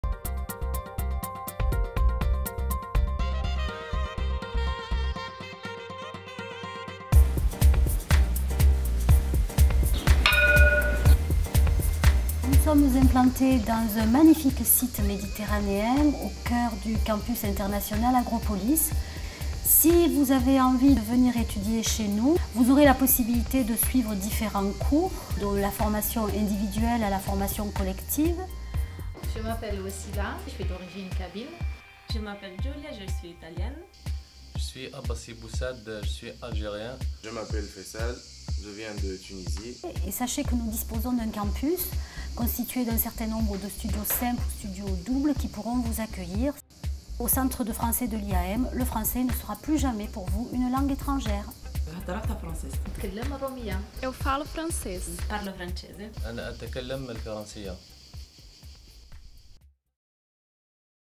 Le Centre de Français Langue Etrangère de l' IAMM
Présentation du Centre de Français Langue Étrangère de l'IAM de Montpellier